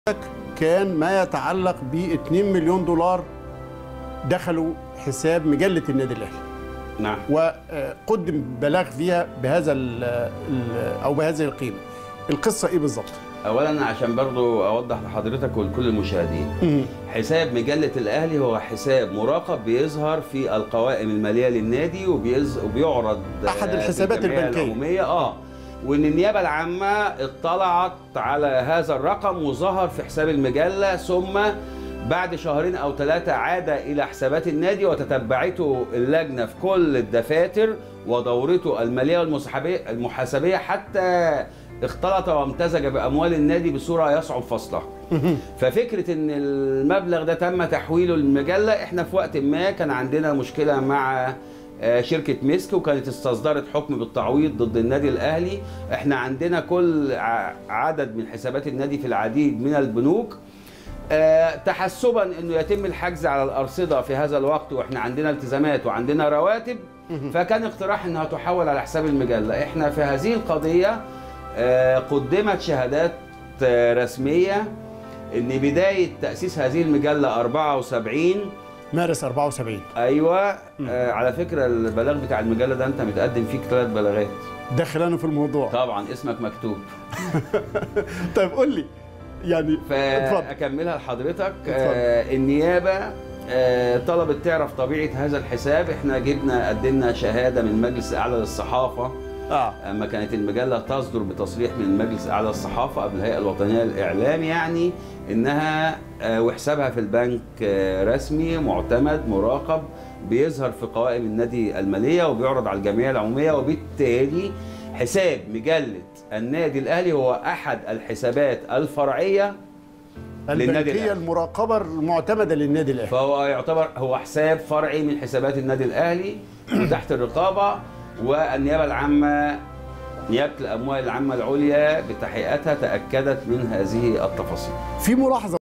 محمد عثمان يكشف حقيقة دخول " أموال " إلى مجلة الاهلي الخاصة بتبرعات " ترك آل شيخ "
ضيف الفقرة
محمد عثمان ـ المستشار القانوني للنادي الأهلي السابق